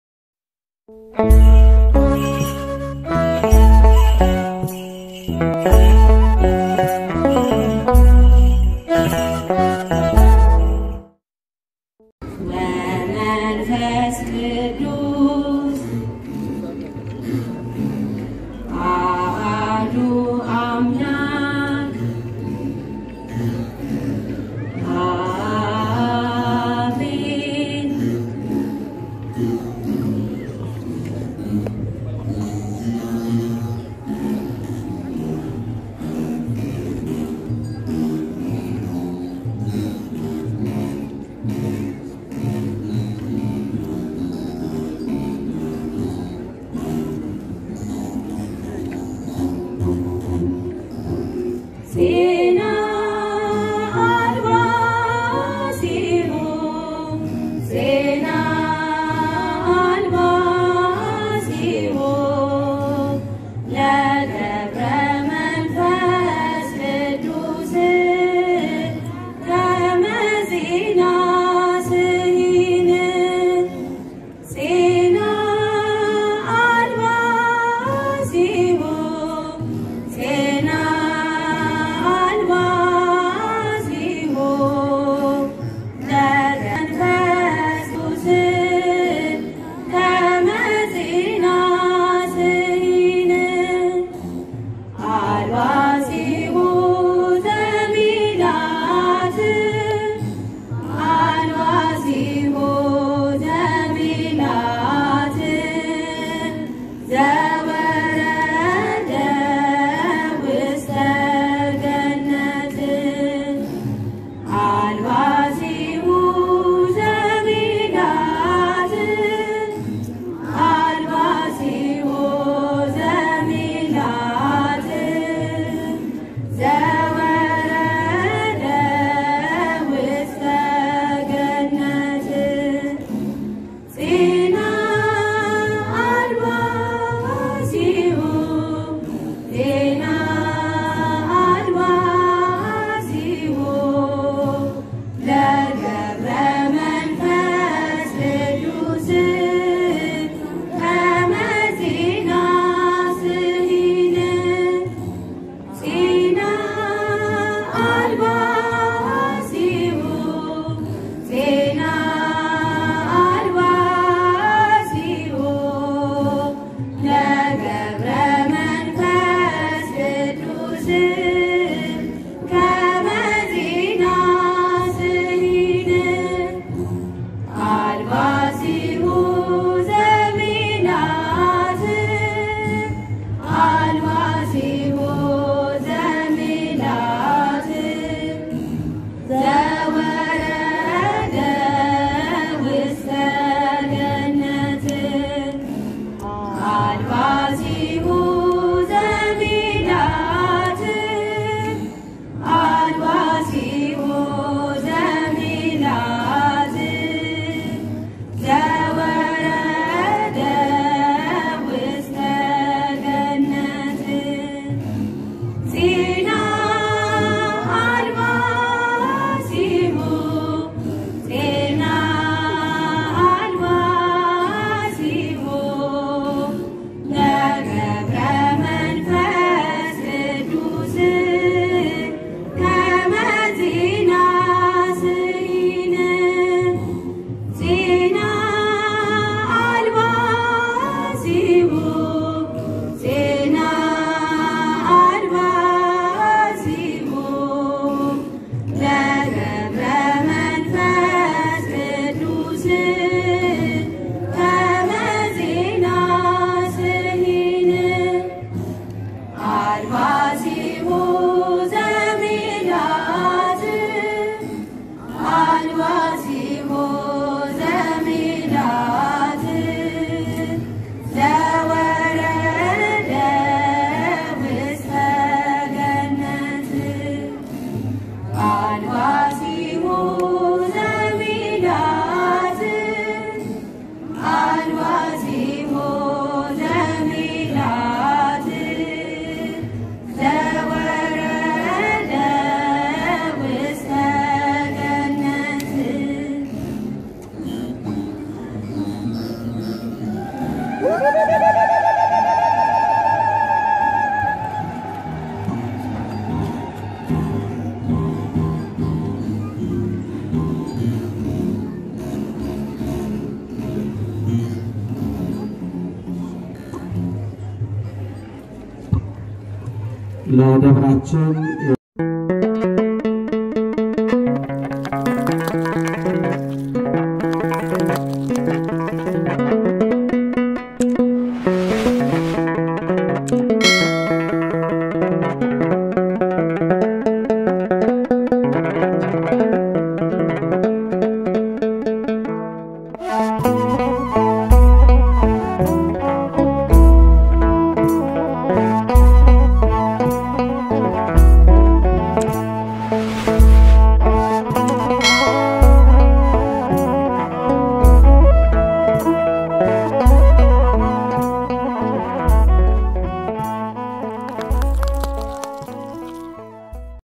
ከህፃን እስከ አዛውንት የተሳተፉበት የበገና መዝሙር በህብረት ሲያቀርቡ Zemari Birhanu Terefe YouTube Share Subscribers like 😍
የጵጥሮስ ወጳውሎስ ሰንበት ተማሪዎች